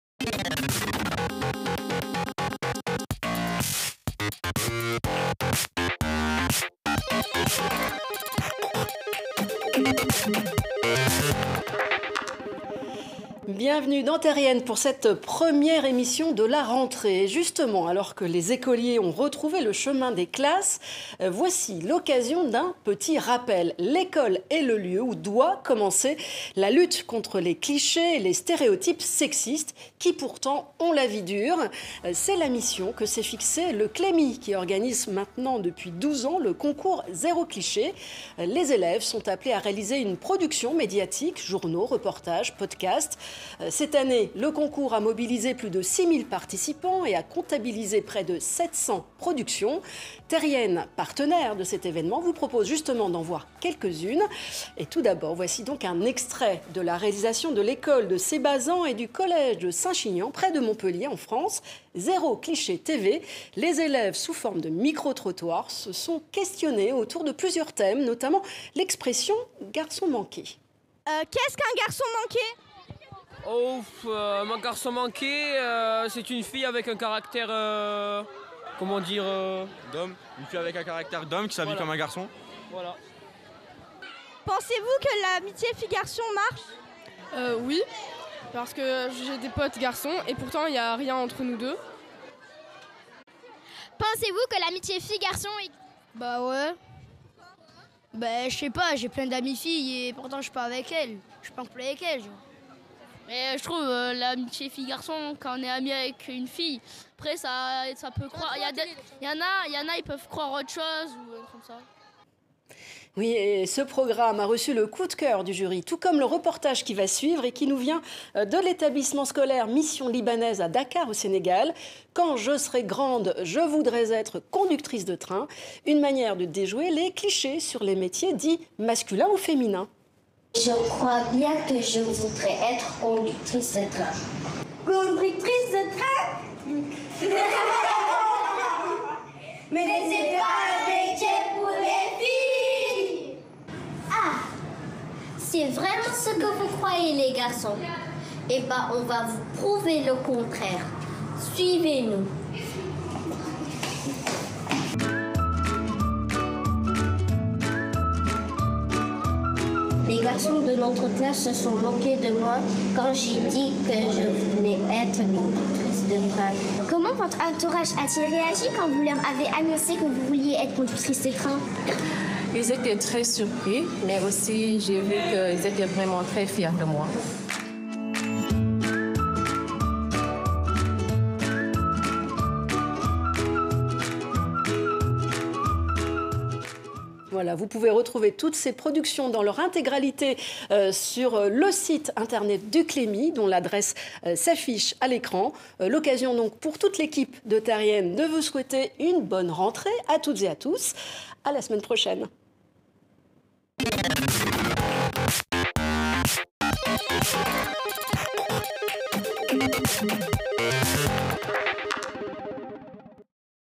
Concours clémi
L'école est le lieu de la lutte contre les stéréotypes sexistes. Depuis 12 ans, @LeCLEMI organise le concours Zéro Cliché, qui récompense une production médiatique réalisée par les élèves. Voici quelques extraits des réalisations primées, avec Terriennes.